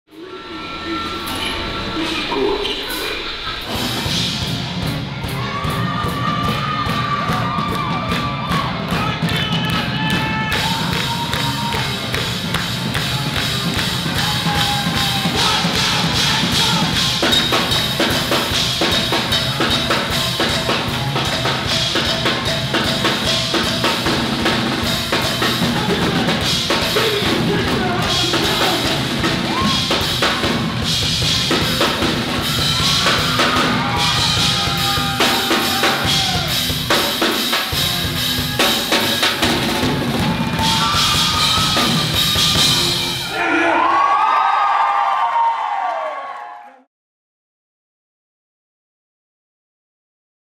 Scorched Waves Live at The Encore 11.02.23 | The Pointer at UWSP
Scorched Waves, a local indie rock band based in Stevens Point, WI, played at The Encore at UWSP on November 2.

The Pointer is the student-run newspaper at the University of Wisconsin-Stevens Point.

Reporting by Abigail Cherek
Cinematography by Will Boeckman